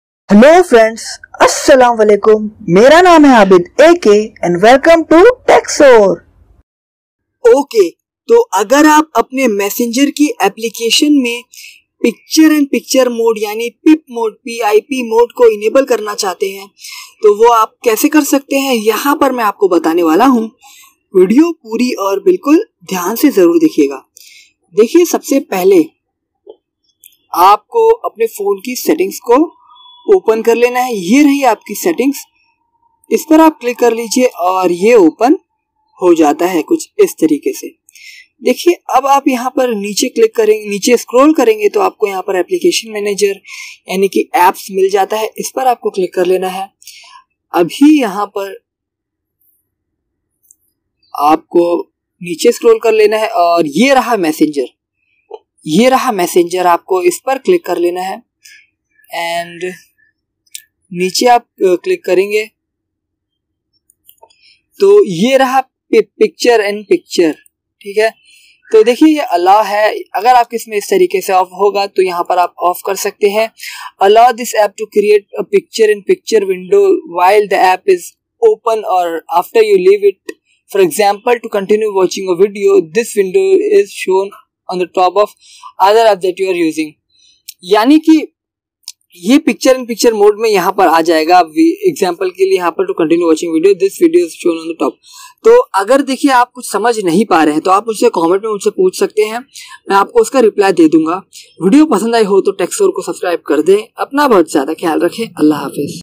How to Access Picture in Picture in Messenger || Android
iss Video me maine bataya hai agar aap apne phone me facebook messenger ki application me picture in picture mode ki setting karna chahte hai to ye video apke liye hai.

Talk soon,
Abid Akay